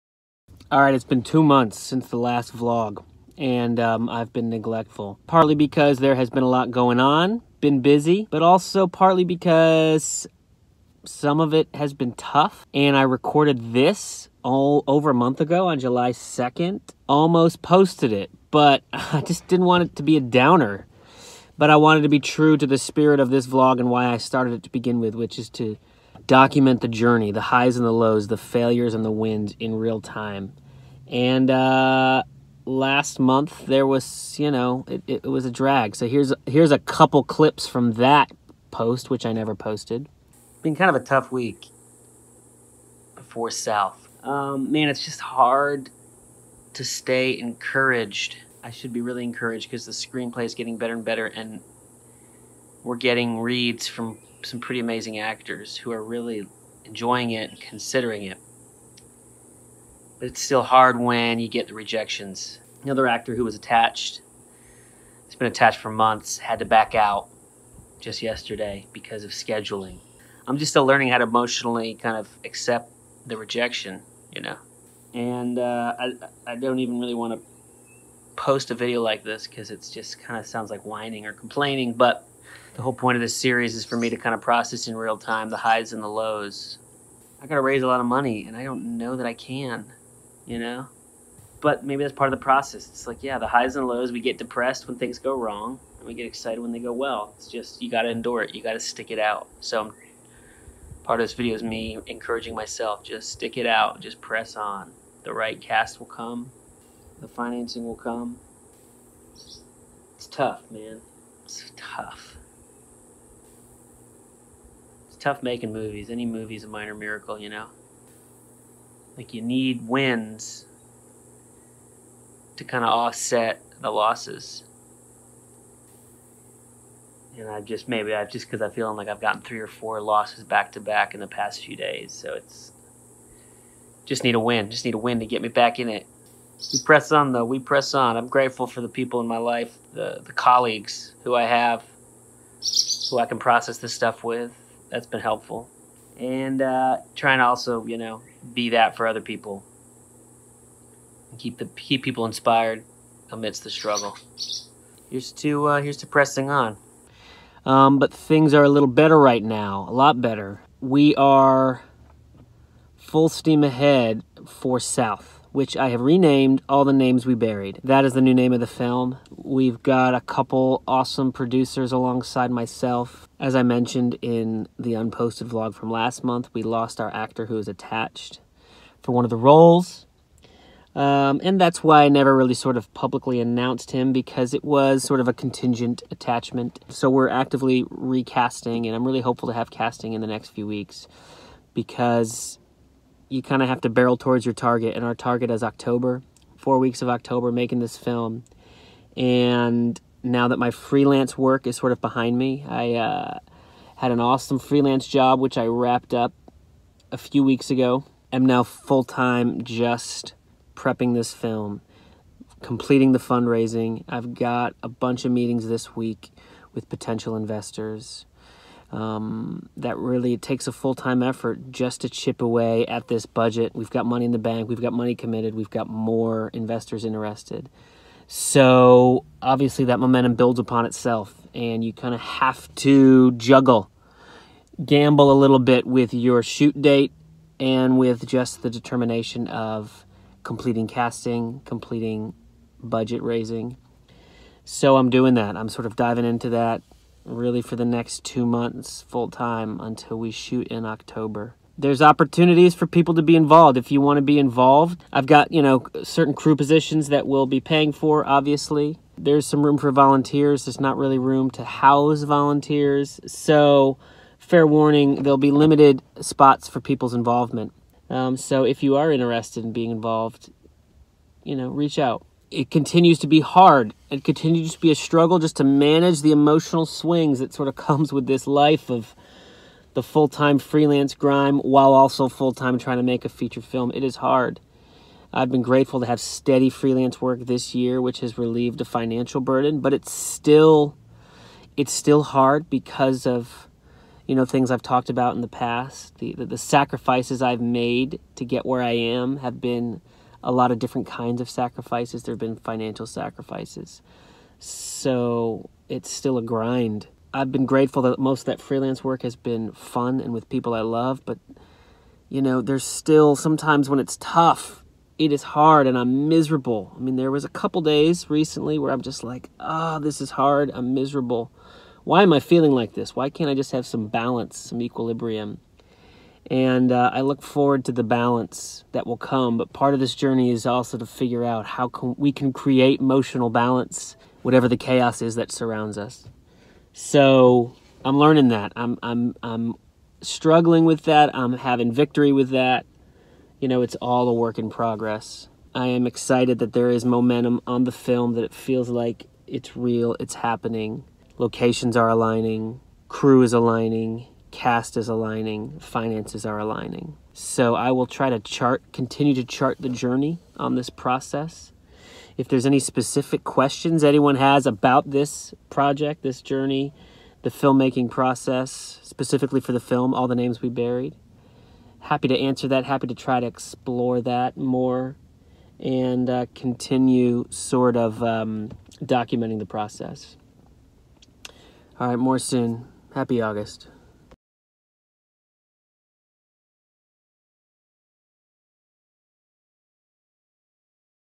FP44 - "Neglectful"
It's been two months since my last vlog. I've been neglectful. Here's an update on the new film, as I continue to process the journey of filmmaking.